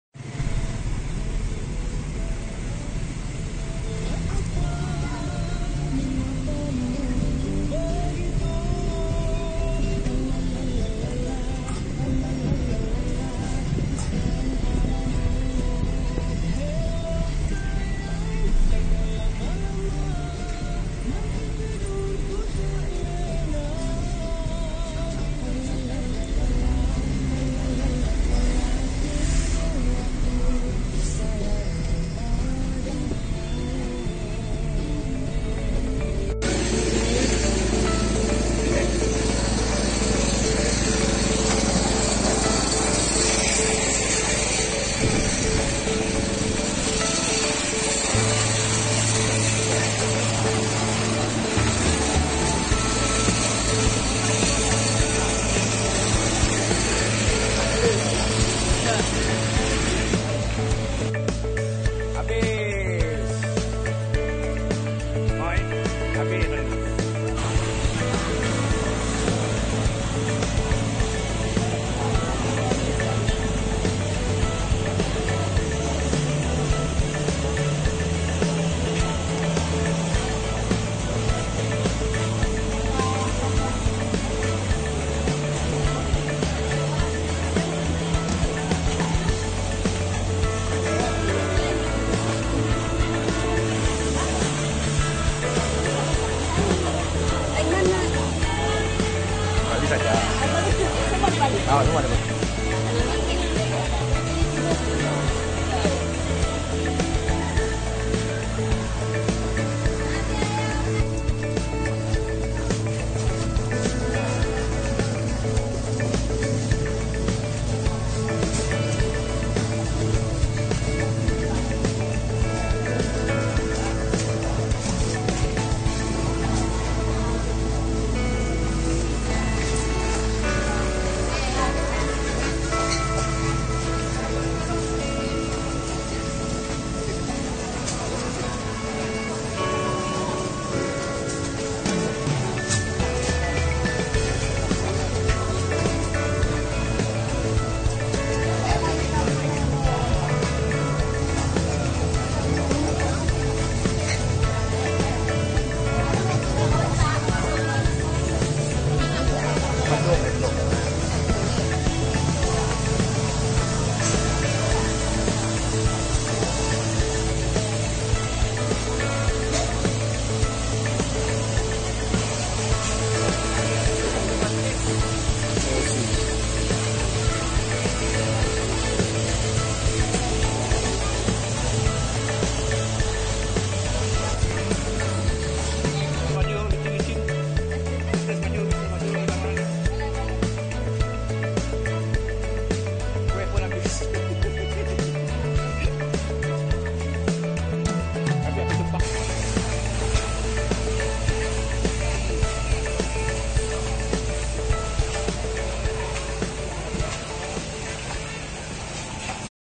Bazaar Ramadhan Pekan Bekenu
Pergi pasar Ramadhan Pekan Bekenu,Sibuti..45km dari bandar Miri.Hari pertama puasa. Lokasi dekat Pasar Tamu Bekenu. Bekenu Ramadhan #2019